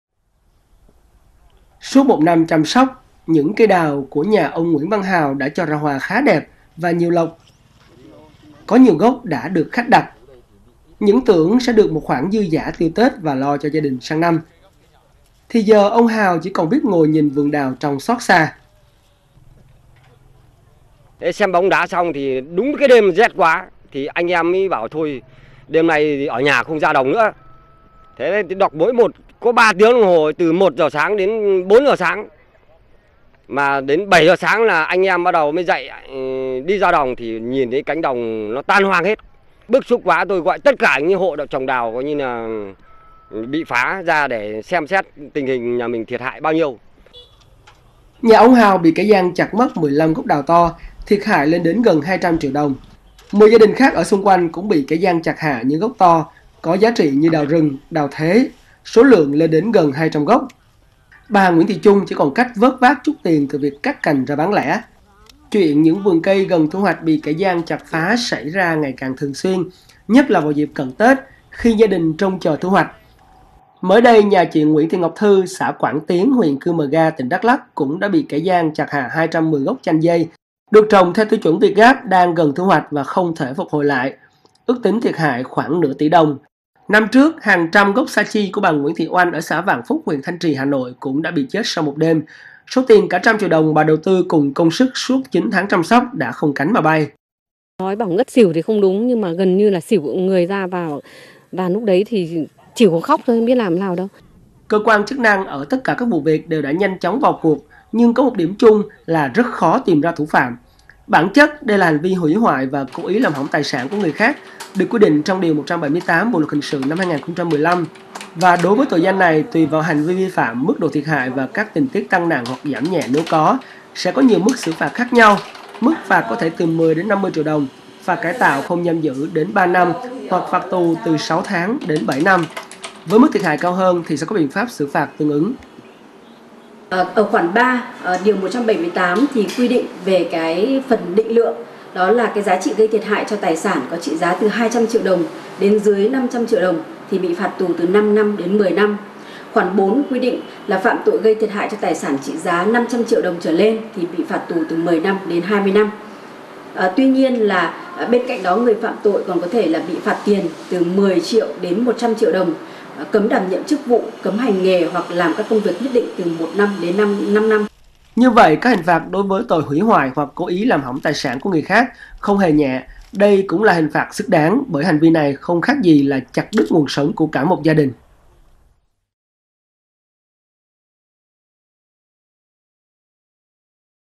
Chặt phá nông sản có thể đi tù trên 10 năm
VTC9 | Những kẻ gian phá hoại, chặt trộm vườn cây có thể sẽ phải đối mặt với hình phạt từ 10 – 20 năm tù.